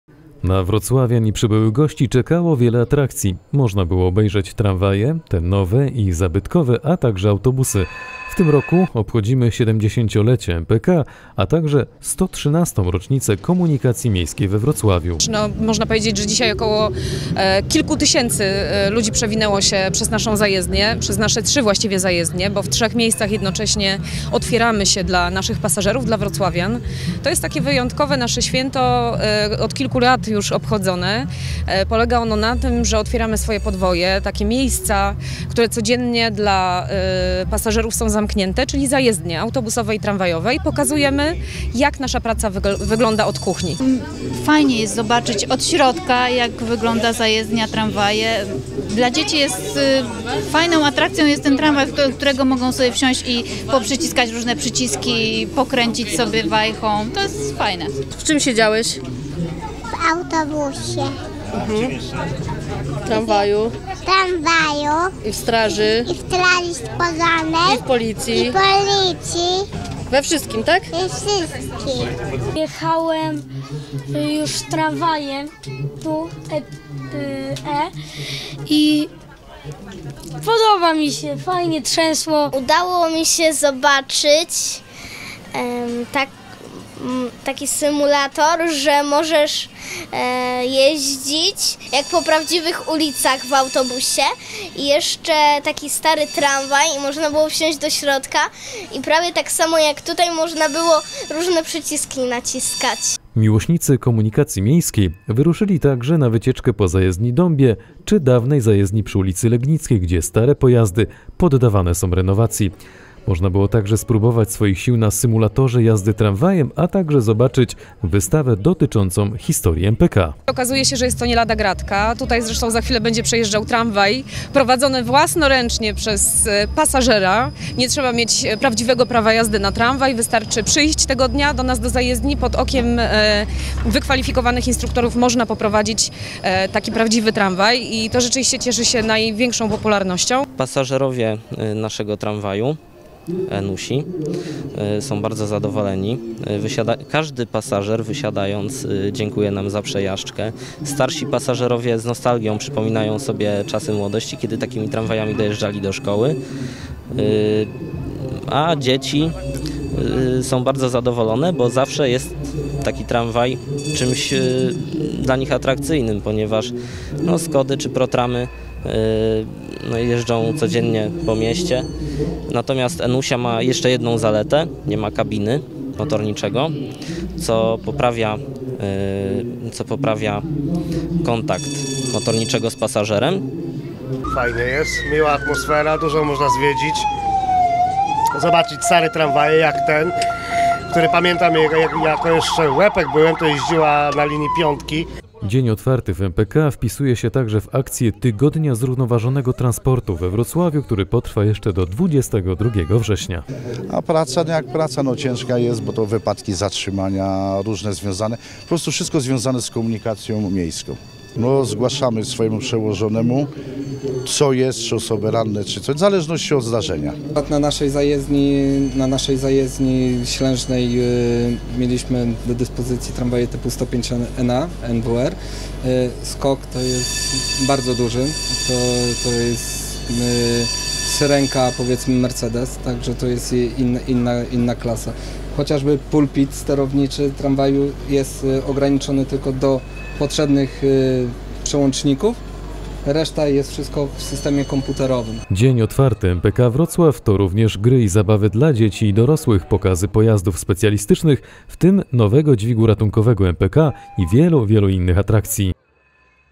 Dzień otwarty MPK Wrocław
Wszyscy, którzy interesują się komunikacją miejską i jej historią oraz ci, którzy marzyli o tym, aby sprawdzić się w roli motorniczego mogli w miniony weekend spełnić swoje marzenia. Dzień otwarty MPK  zorganizowany został na terenie Zajezdni przy ul. Powstańców Śląskich.